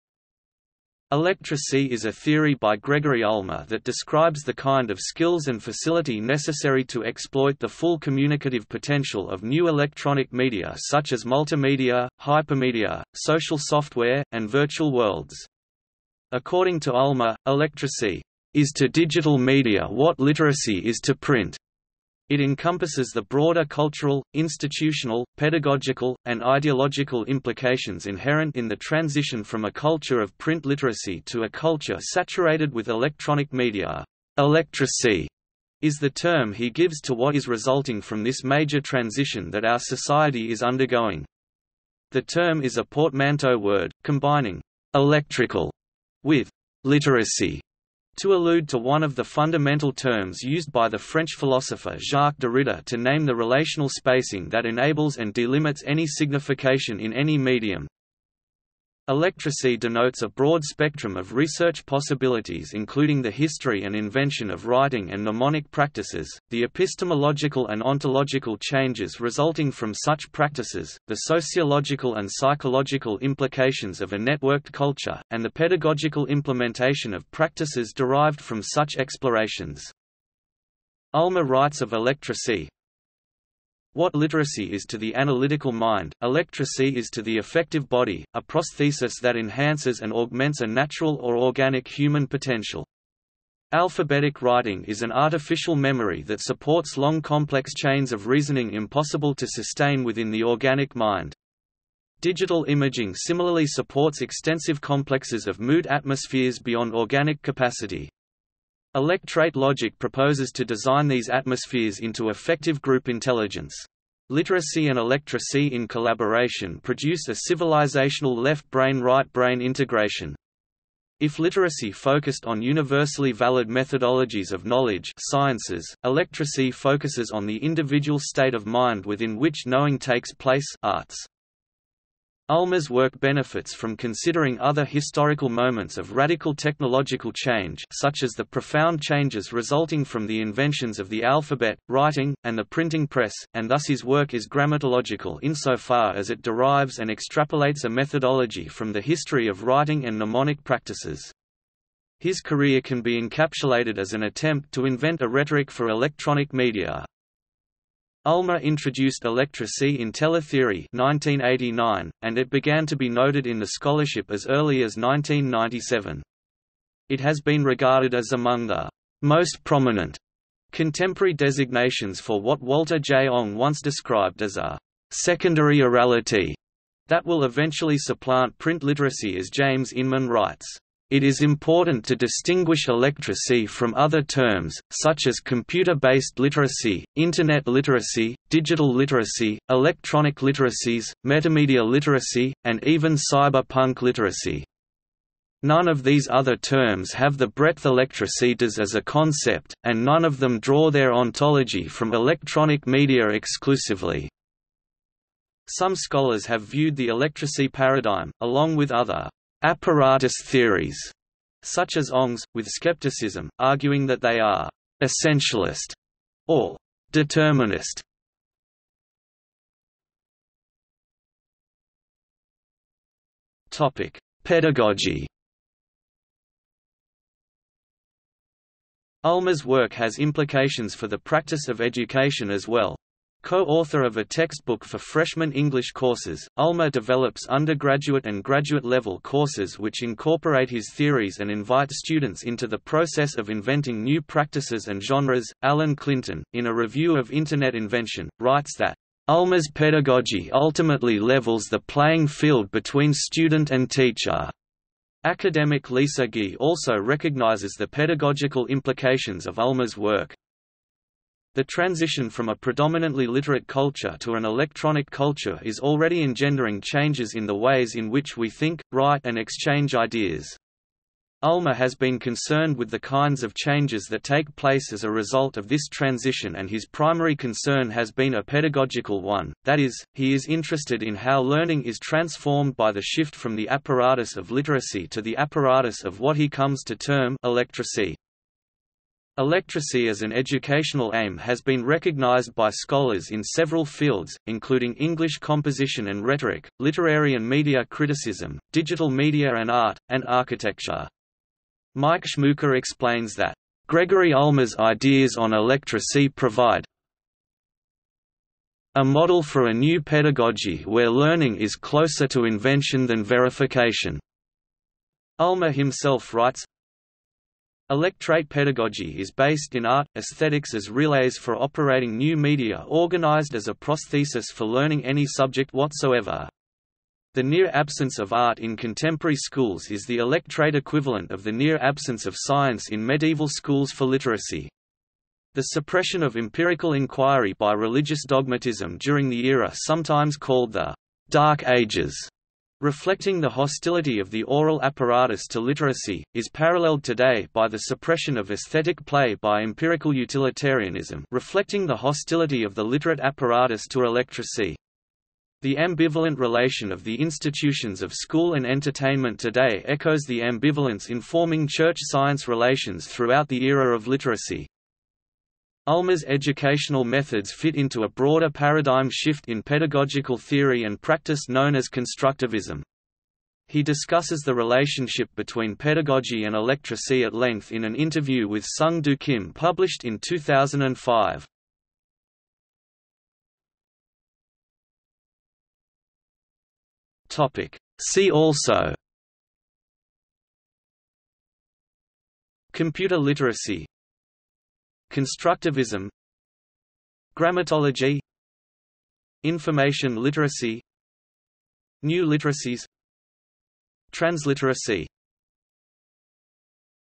Electracy | Wikipedia audio article
00:03:39 Pedagogy
00:06:23 See also

- Socrates

SUMMARY
Electracy is a theory by Gregory Ulmer that describes the kind of skills and facility necessary to exploit the full communicative potential of new electronic media such as multimedia, hypermedia, social software, and virtual worlds.  According to Ulmer, electracy "is to digital media what literacy is to print."  It encompasses the broader cultural, institutional, pedagogical, and ideological implications inherent in the transition from a culture of print literacy to a culture saturated with electronic media. "Electracy" is the term he gives to what is resulting from this major transition that our society is undergoing.  The term is a portmanteau word, combining "electrical" with "literacy", to allude to one of the fundamental terms used by the French philosopher Jacques Derrida to name the relational spacing that enables and delimits any signification in any medium.
Electracy denotes a broad spectrum of research possibilities including the history and invention of writing and mnemonic practices, the epistemological and ontological changes resulting from such practices, the sociological and psychological implications of a networked culture, and the pedagogical implementation of practices derived from such explorations.
Ulmer writes of electracy:

What literacy is to the analytical mind, electracy is to the affective body: a prosthesis that enhances and augments a natural or organic human potential. Alphabetic writing is an artificial memory that supports long complex chains of reasoning impossible to sustain within the organic mind. Digital imaging similarly supports extensive complexes of mood atmospheres beyond organic capacity. Electrate logic proposes to design these atmospheres into affective group intelligence. Literacy and electracy in collaboration produce a civilizational left-brain right-brain integration. If literacy focused on universally valid methodologies of knowledge (sciences), electracy focuses on the individual state of mind within which knowing takes place (arts).

Ulmer's work benefits from considering other historical moments of radical technological change (such as the profound changes resulting from the inventions of the alphabet, writing, and the printing press), and thus his work is grammatological insofar as it derives and extrapolates a methodology from the history of writing and mnemonic practices.  His career can be encapsulated as an attempt to invent a rhetoric for electronic media.
Ulmer introduced electracy in Teletheory (1989), and it began to be noted in the scholarship as early as 1997.  It has been regarded as among the "most prominent" contemporary designations for what Walter J. Ong once described as a "secondary orality" that will eventually supplant print literacy As James Inman writes, "It is important to distinguish electracy from other terms, such as computer-based literacy, Internet literacy, digital literacy, electronic literacies, metamedia literacy, and even cyber-punk literacy. None of these other terms have the breadth electracy does as a concept, and none of them draw their ontology from electronic media exclusively". Some scholars have viewed the electracy paradigm, along with other "apparatus theories" such as Ong's, with skepticism, arguing that they are "essentialist" or "determinist".